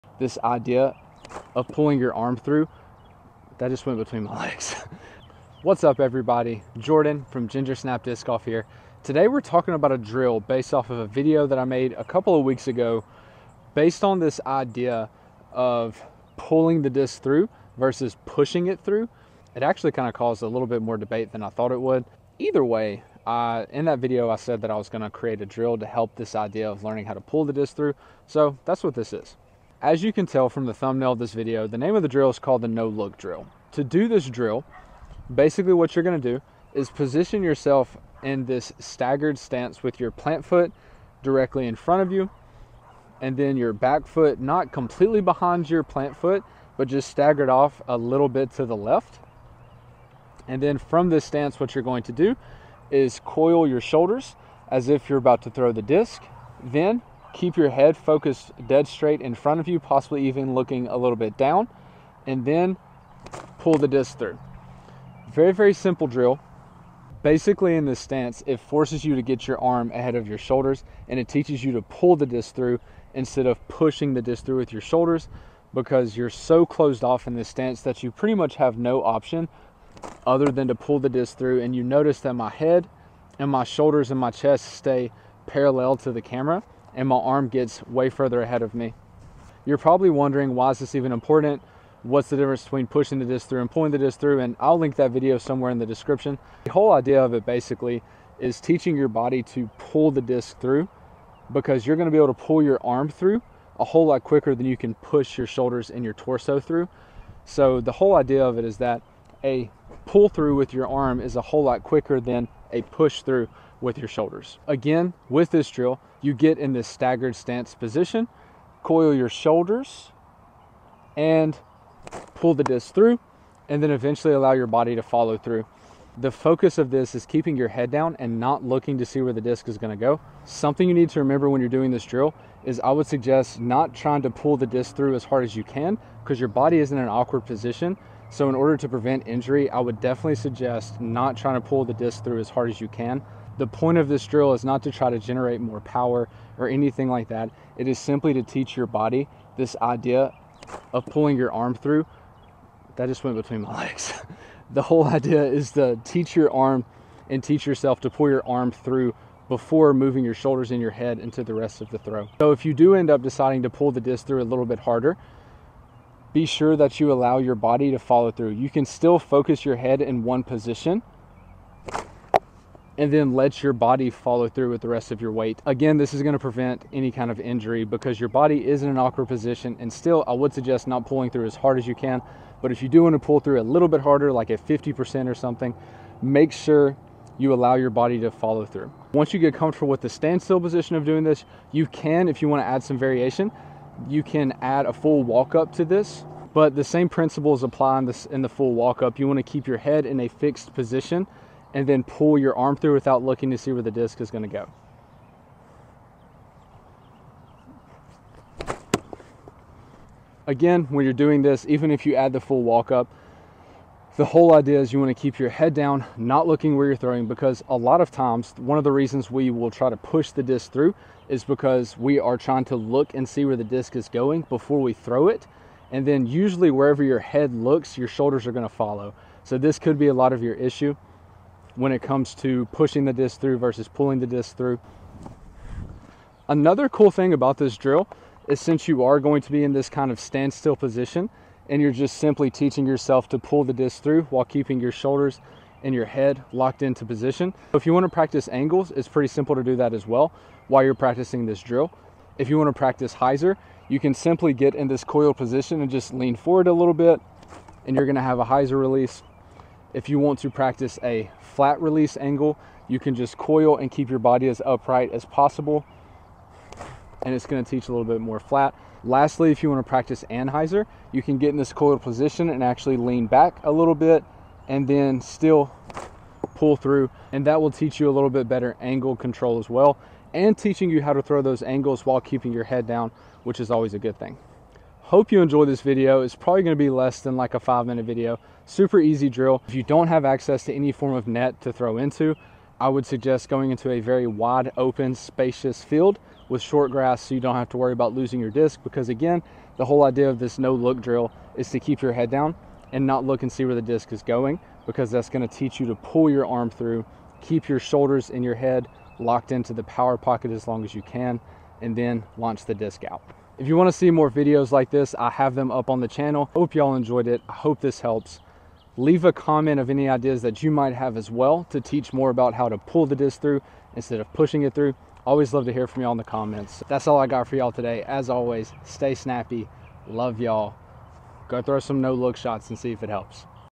Fix Your Pull Through & Gain Distance With This Drill!
This simple drill will help you smooth out your pull through and gain more distance with less effort!